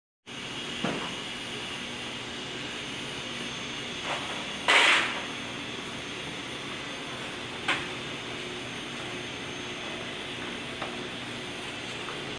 Eggs Vacuum Egg Suction Lifter Machine / Vacuum Egg Lifter
Moving the eggs from egg cartons to the machine

The lifter, with the use of rubber suction cups and accumulated vacuum, grabs the eggs which allow for their transportation.

Application： bakeries, confectioneries, pasta making companies, egg processing companies

Characters:

1. Fast and safe egg transfer from egg flat to setter tray
2. Eliminates the risk of egg damage
3. Ergonomic and lightweight construction
4. Easy to clean